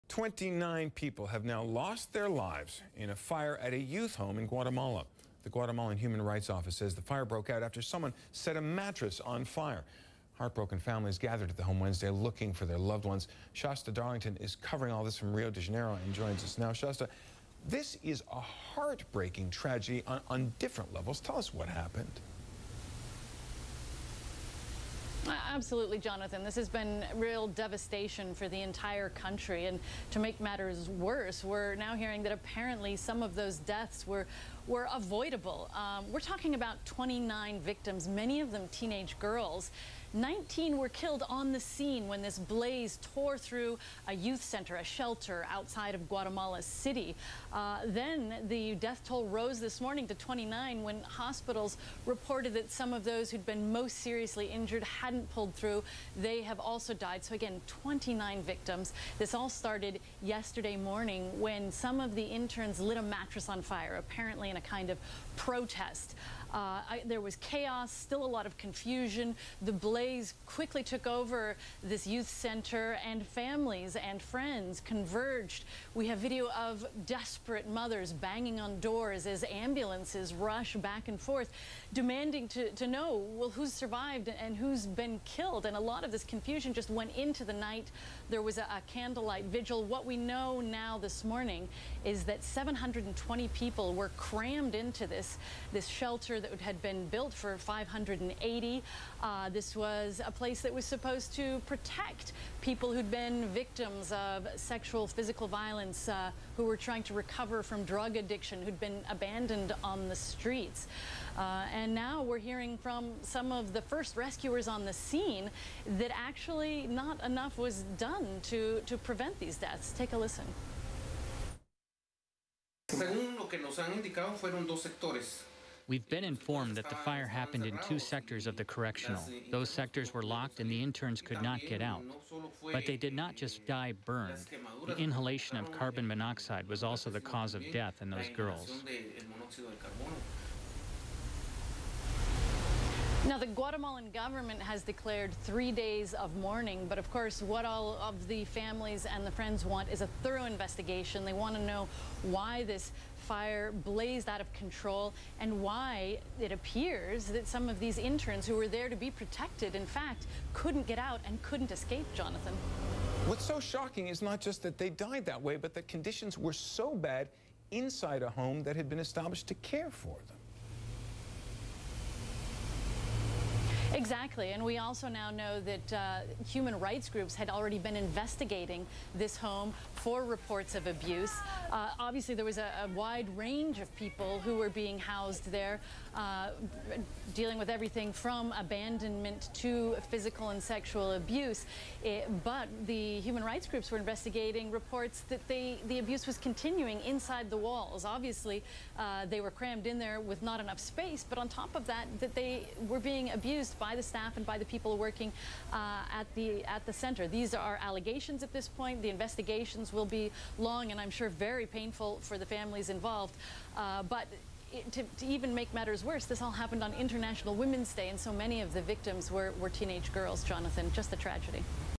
Youth center fire kills dozens, mostly teen girls
A fire at a youth home in Guatemala claims dozens of lives. CNN's Shasta Darlington reports.